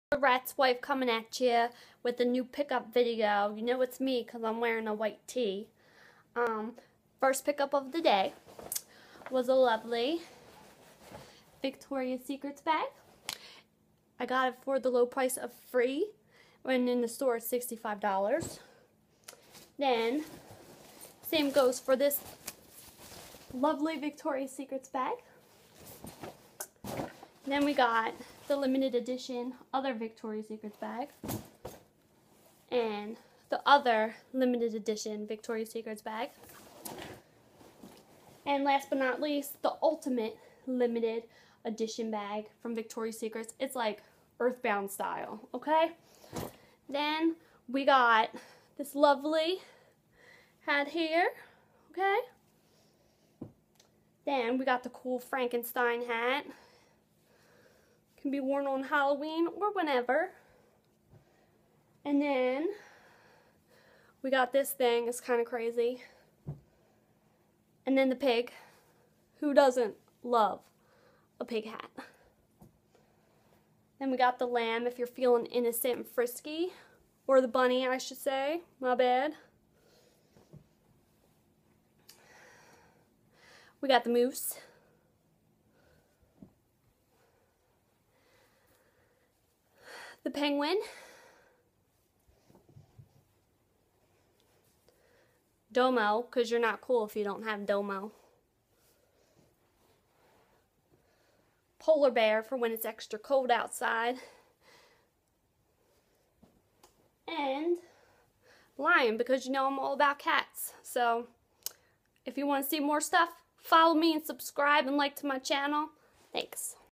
My Wife's 1st Pick Up Video ( also making fun of my cloths)
This is my Wife's 1st pick up video, She was feeling a little silly and made it. Also decided to take a stab at me always wearing a white T-Shirt just like Marrierguygamer.